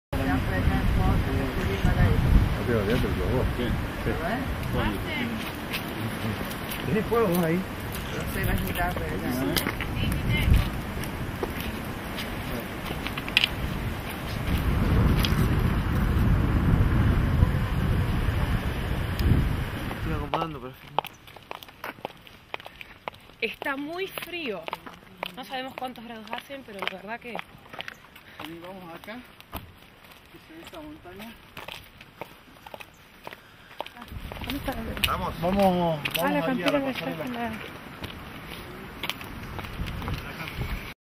Miss Mundo Argentina, Imágenes de Muestra. // Miss World Argentina, test footage
Test footage from Miss World Argentina. // Imágenes de Muestra de la grabación de Miss Mundo Argentina en el Glaciar Perito Moreno, Argentina, Noviembre de 2009.